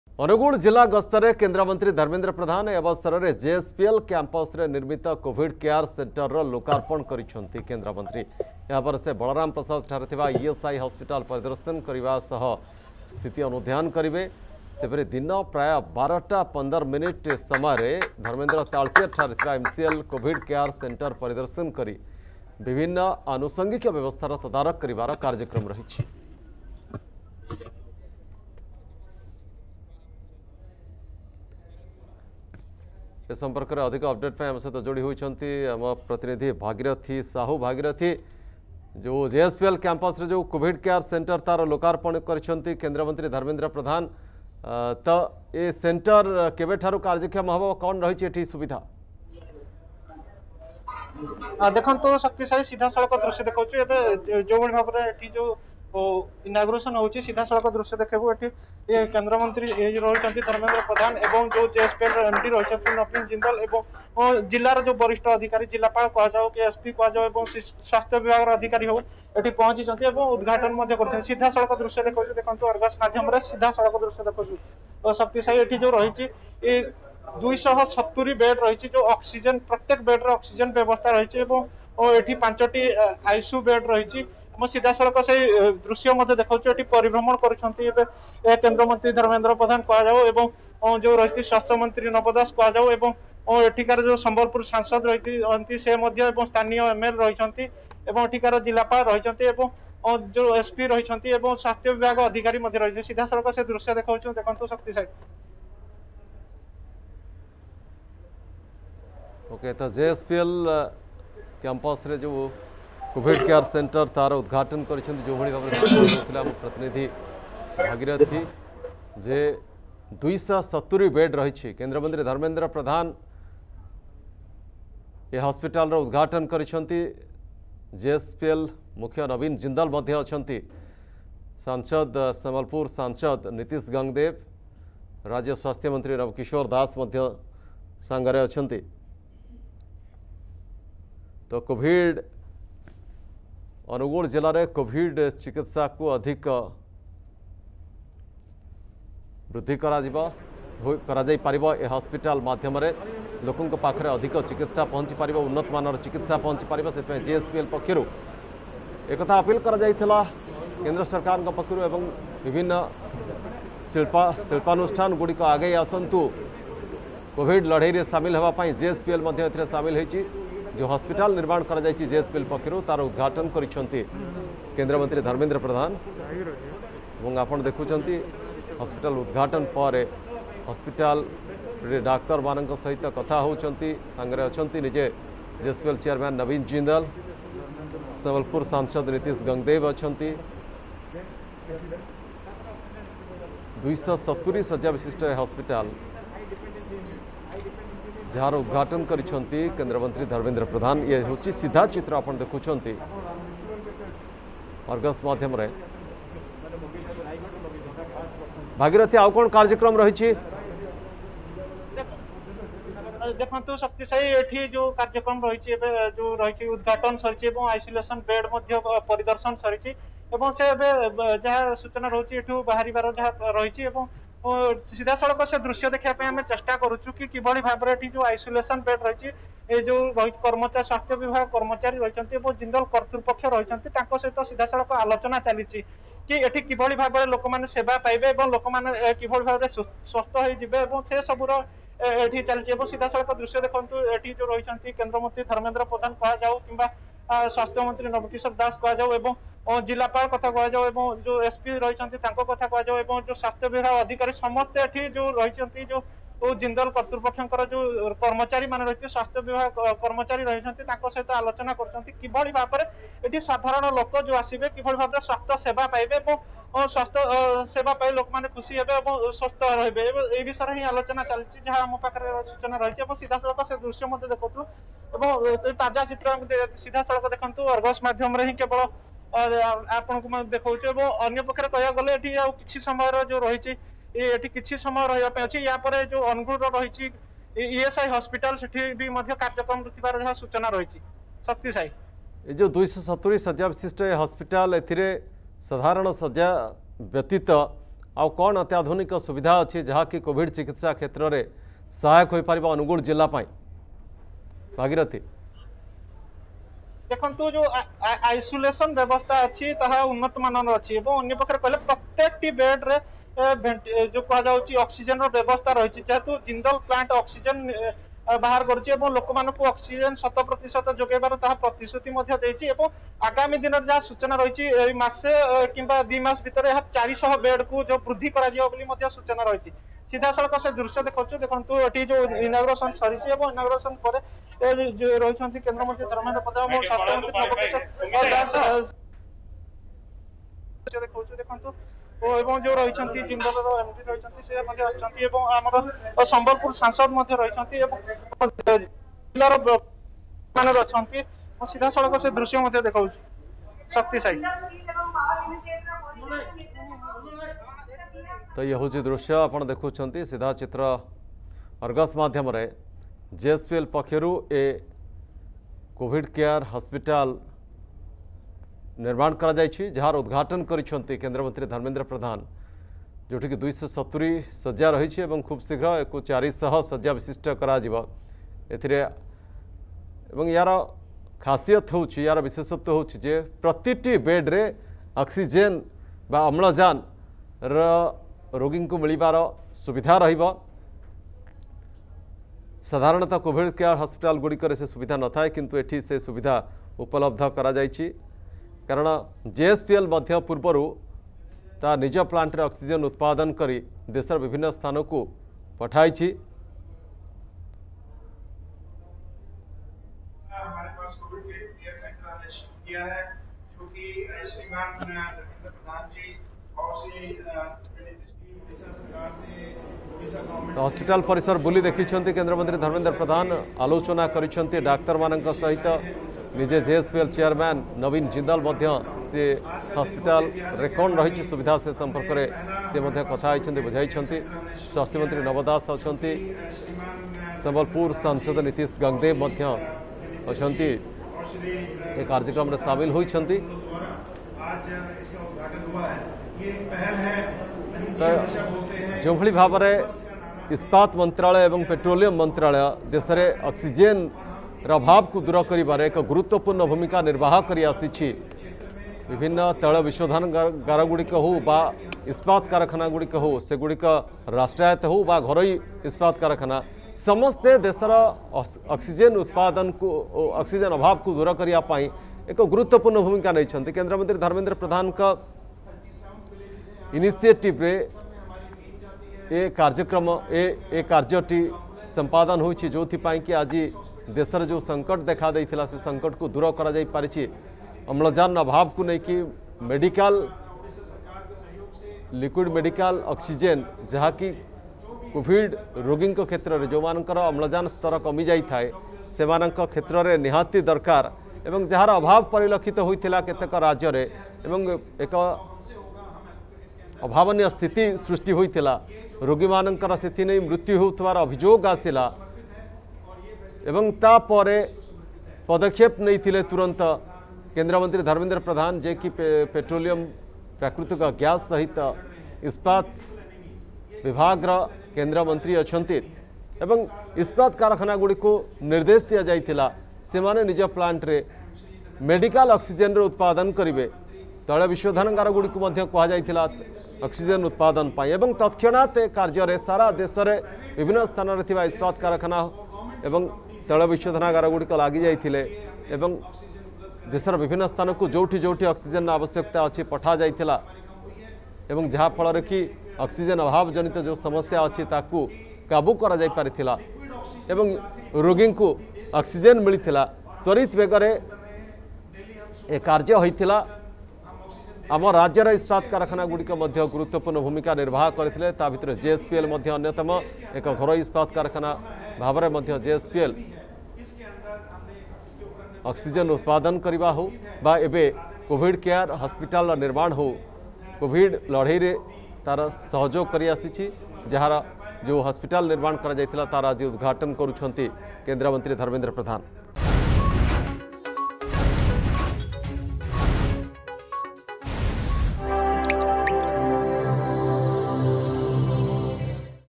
ଅନୁଗୁଳ ଜିଲ୍ଲା ଗସ୍ତରେ କେନ୍ଦ୍ରମନ୍ତ୍ରୀ ଧର୍ମେନ୍ଦ୍ର ପ୍ରଧାନ
ଜେଏସପିଏଲ କ୍ୟାମ୍ପସରେ ନିର୍ମିତ କୋଭିଡ କେୟାର ସେଣ୍ଟରକୁ ଲୋକାର୍ପଣ କରିଛନ୍ତି କେନ୍ଦ୍ରମନ୍ତ୍ରୀ । ଏହାପରେ ସେ ବଳରାମ ପ୍ରସାଦ ଠାରେ ଥିବା ଇଏସଆଇ ହସ୍‌ପିଟାଲ ପରିଦର୍ଶନ କରିବା ସହ ସ୍ଥିତି ଅନୁଧ୍ୟାନ କରିବେ